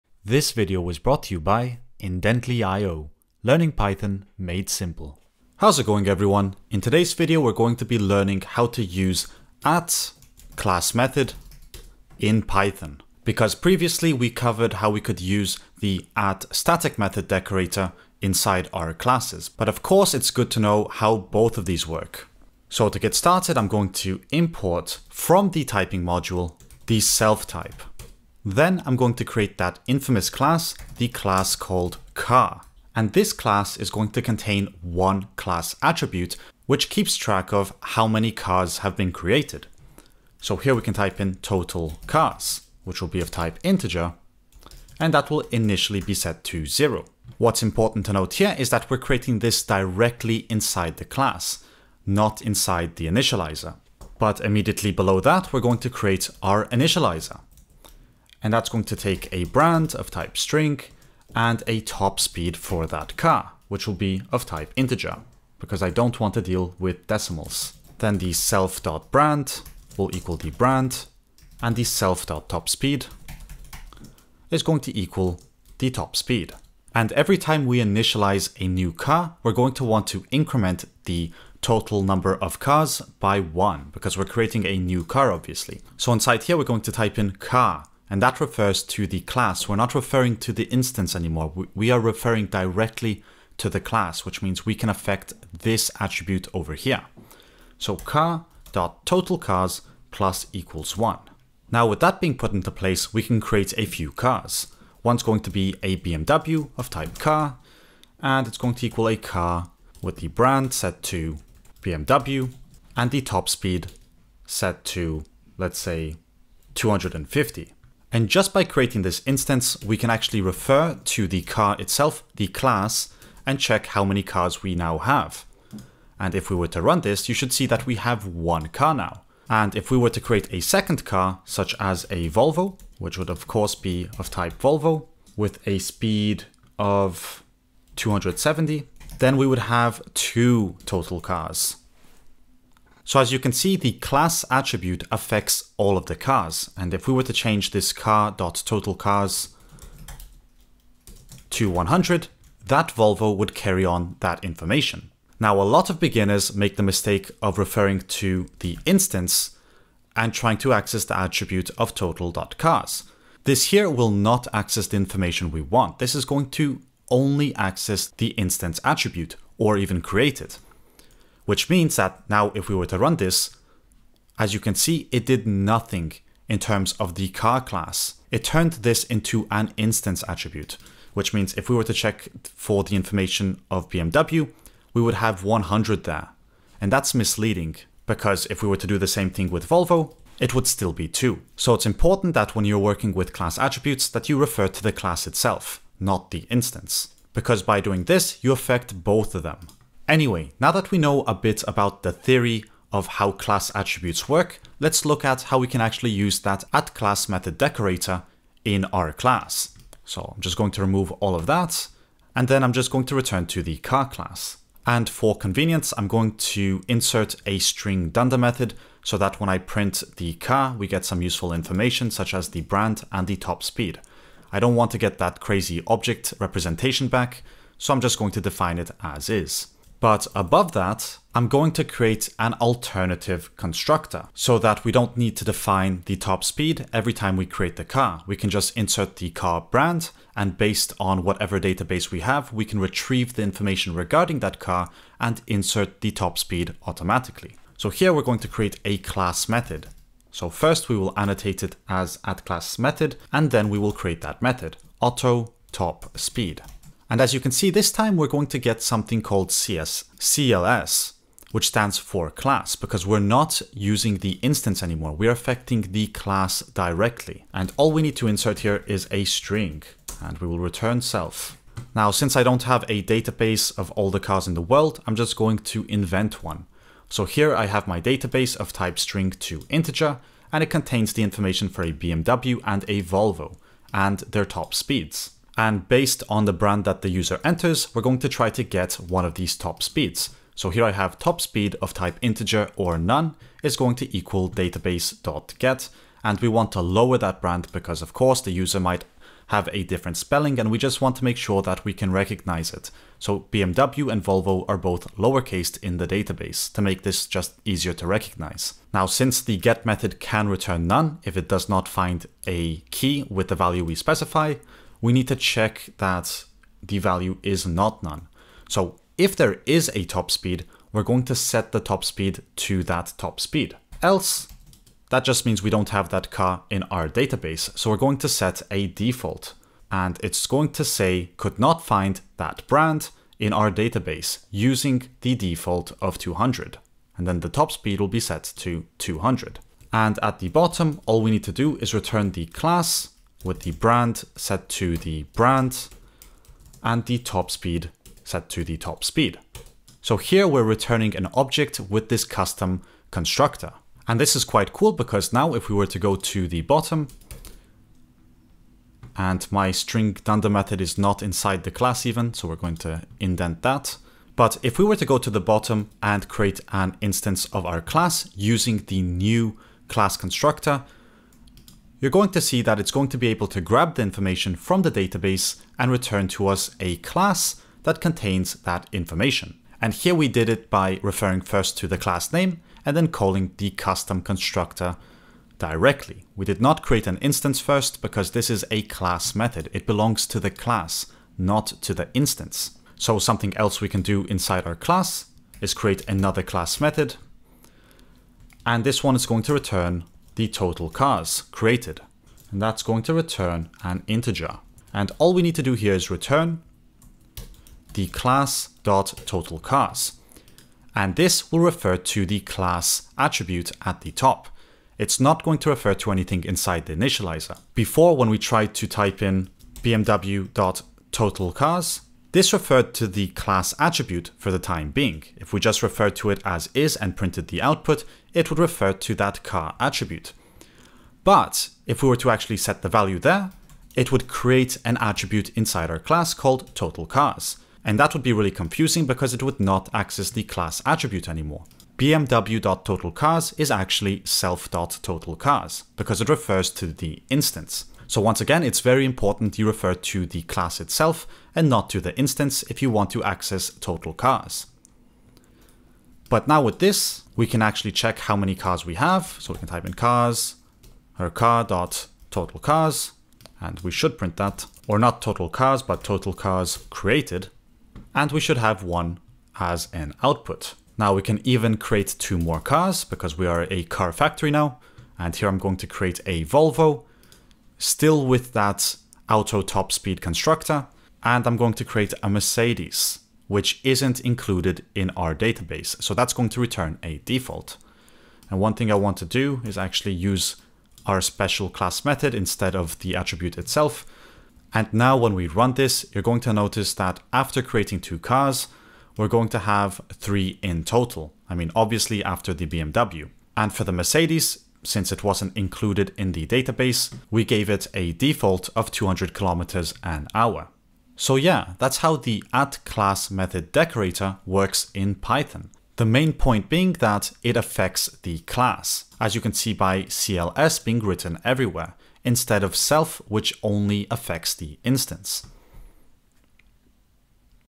explained in Python
In this video I'm going to be teaching you how to use @classmethod in Python. As scary as it might look, it's very useful!